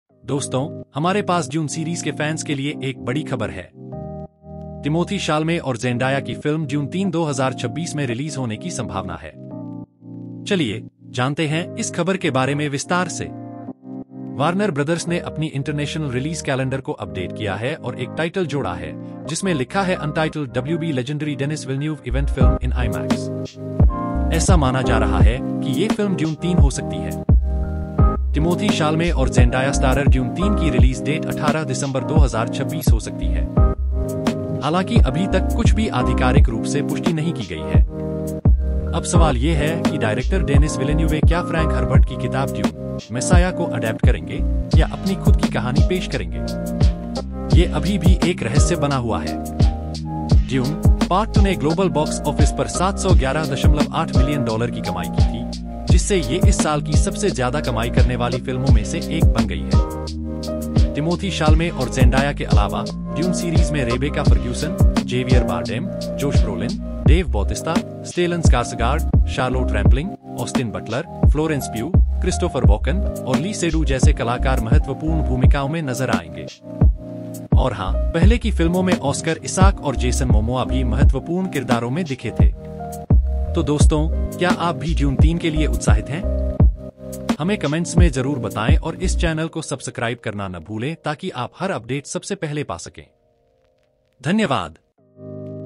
Timothee Chalamet's की 'Dune 3' 2026 में होगी रिलीज !
टिमोथी शालमे और ज़ेंडाया स्टारर 'Dune 3' की रिलीज की तारीख 2026 में होने की संभावना है। वार्नर ब्रदर्स ने अपने इंटरनेशनल रिलीज कैलेंडर को अपडेट किया है, जिसमें 'Untitled WB/Legendary/Denis Villeneuve Event Film in IMAX' का उल्लेख है। माना जा रहा है कि यह फिल्म 'Dune 3' हो सकती है, जिसकी संभावित रिलीज डेट 18 दिसंबर 2026 है। हालांकि, अभी तक कुछ भी आधिकारिक तौर पर पुष्टि नहीं की गई है। डेनिस विलेन्यूवे फिल्म का निर्देशन करेंगे, और यह देखना बाकी है कि वे फ्रैंक हर्बर्ट की 'Dune: Messiah' को अडैप्ट करेंगे या अपनी खुद की कहानी पेश करेंगे।